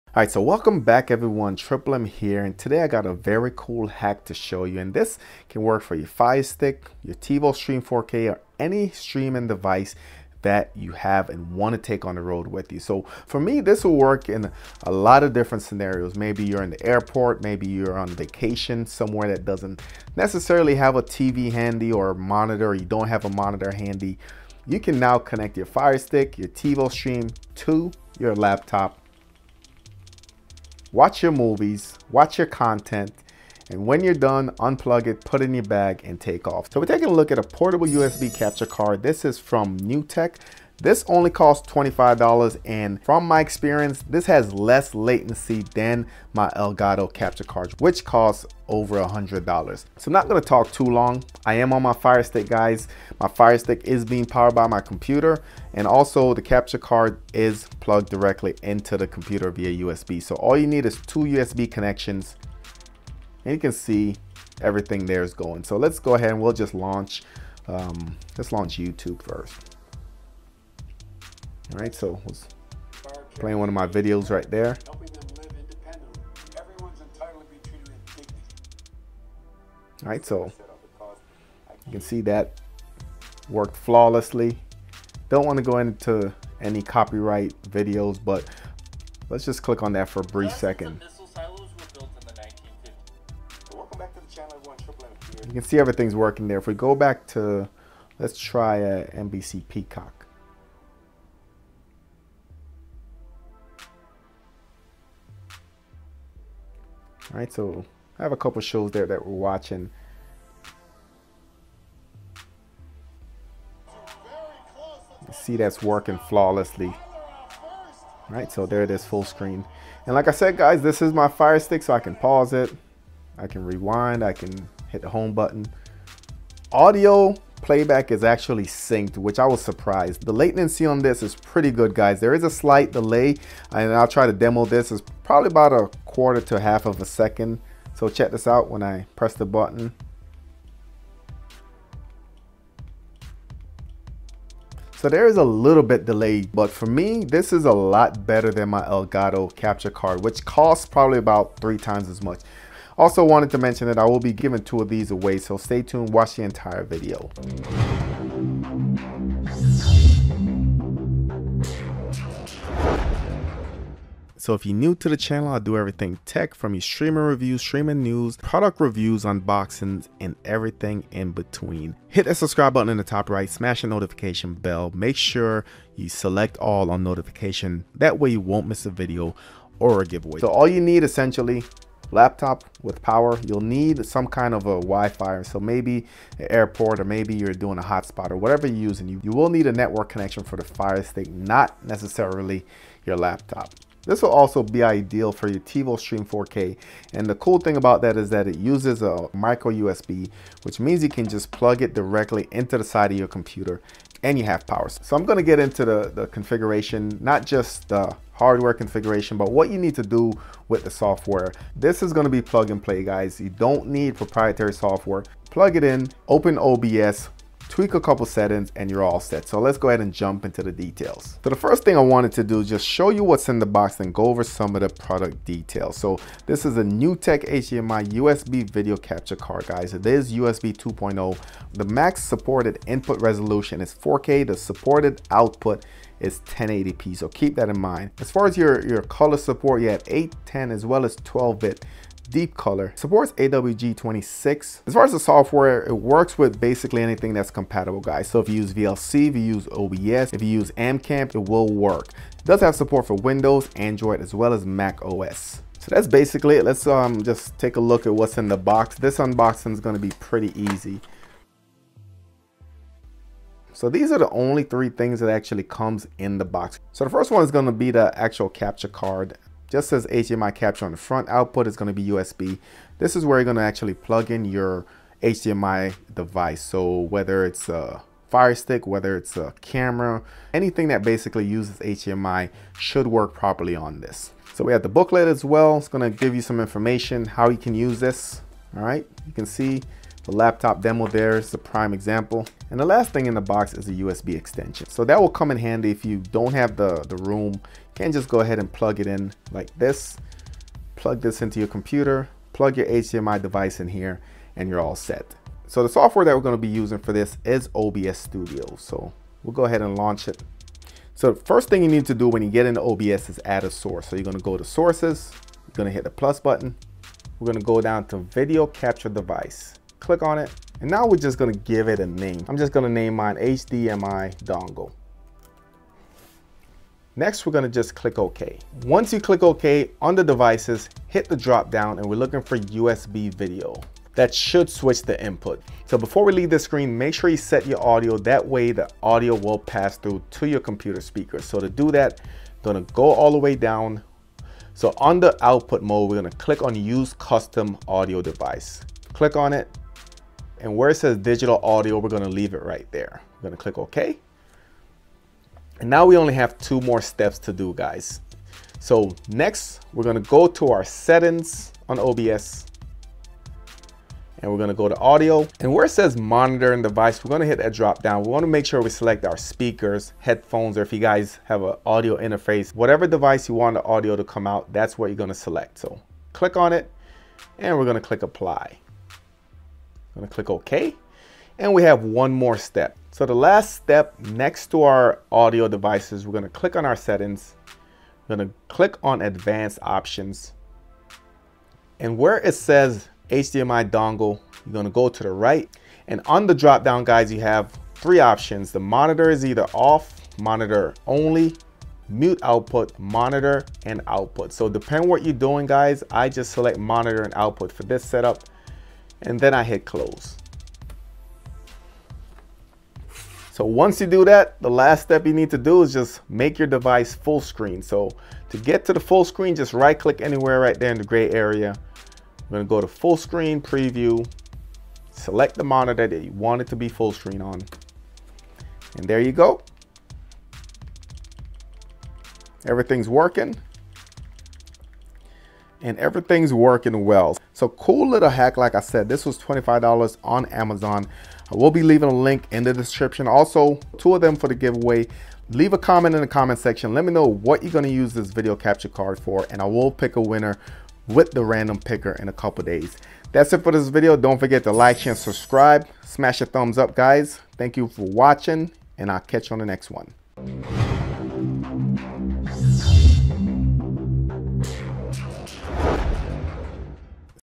Ultimate Firestick Hack | Run your Firestick on your Laptop, Mac, PC!!!
This Hack allows you to run your Amazon Firestick, Tivo Stream 4k, and other HDMI/Streaming Products on your Laptop or PC!!
$25 Capture Card can Be Purchased on Amazon

Jump to areas of interest:
0:00 Intro / Giveaway info
3:00 What do you need?
3:40 Specifications
4:17 What's in the box
5:05 Software Setup